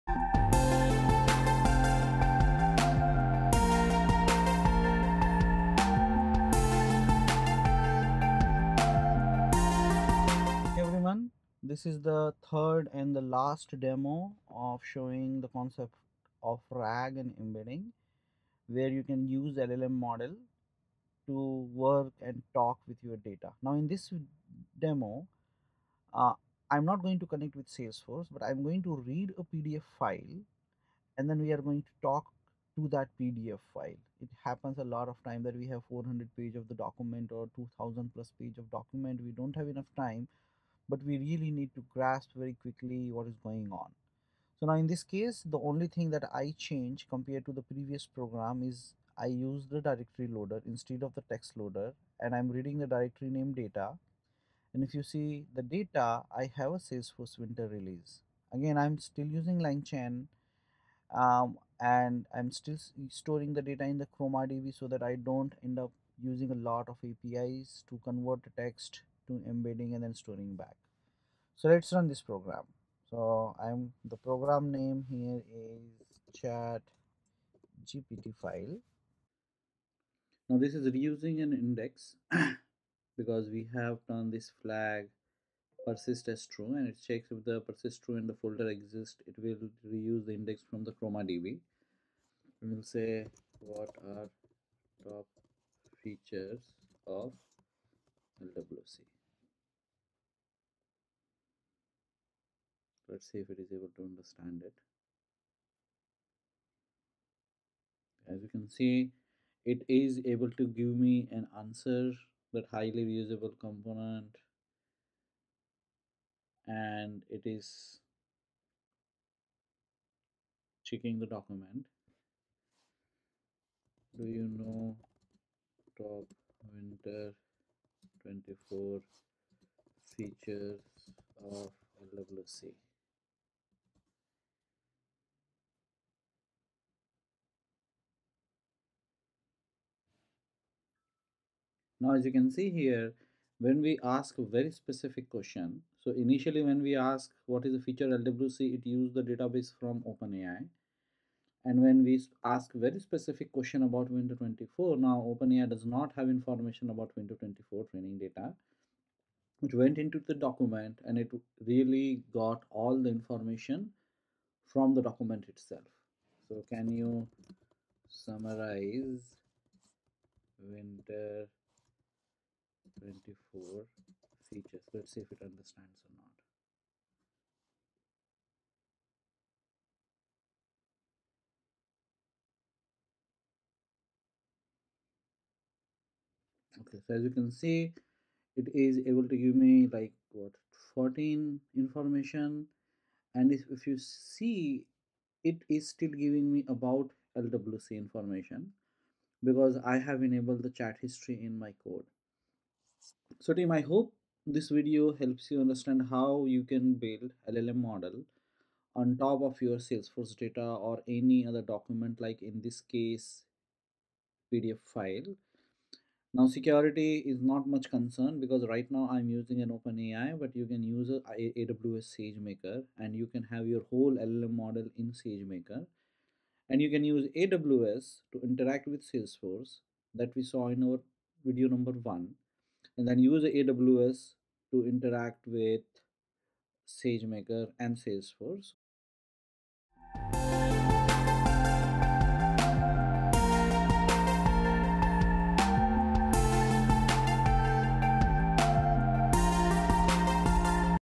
Salesforce RAG Video 3
This is the third & last video in the LLM series to showcase how we can use RAG to provide context to LLM models and talk to our enterprise data, in this case, Salesforce Release Notes